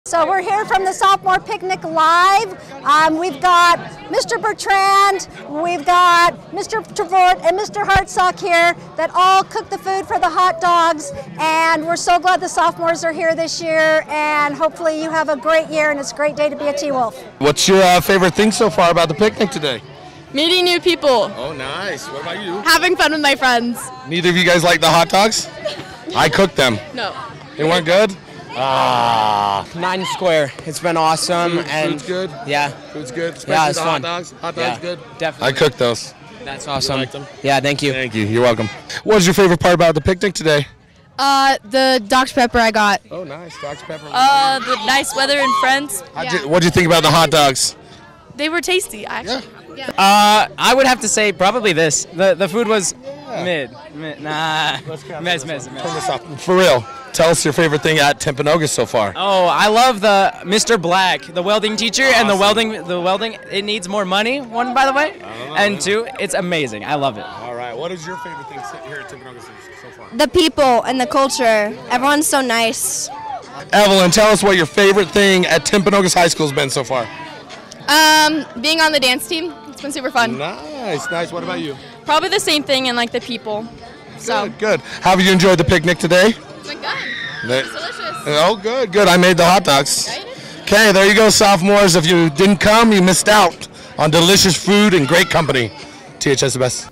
2024 Sophomore Picnic Highlights
Highlights of the 2024 THS Sophomore Picnic held at the football field Wednesday, 4 September, 2024.